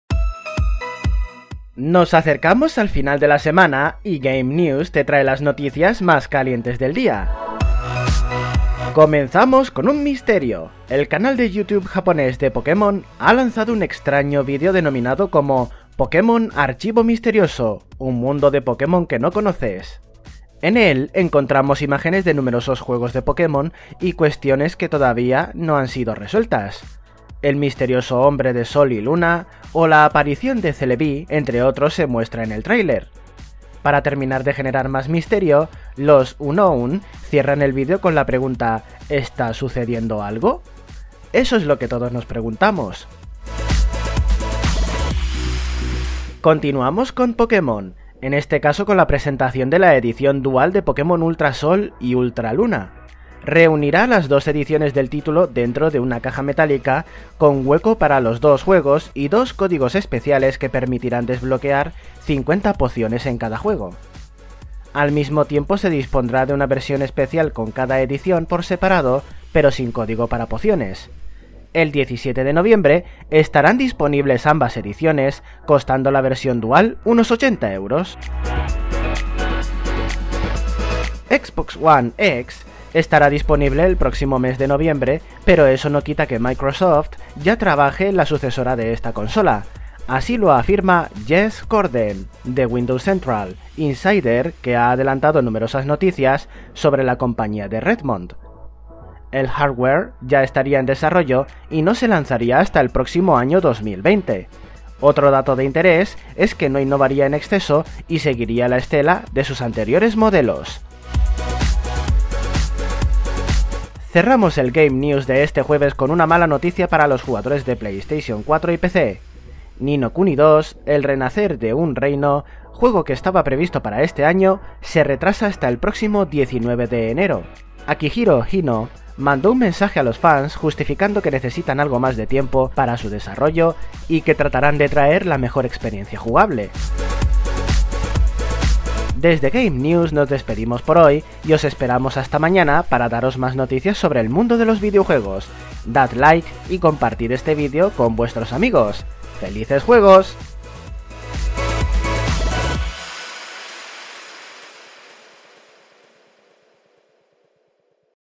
Game News - Pokémon se envuelve en misterio + INFO 13/07/2017
Los Pokémon han sembrado de intrigas el Game News de hoy con un teaser especial de la página oficial de Pokémon JP, mientras se hace realidad la versión dual de Pok´emon Ultrasol y Ultraluna. Microsoft ha empezado a trabajar en su futura consola y Ni no Kuni II se nos va a 2018.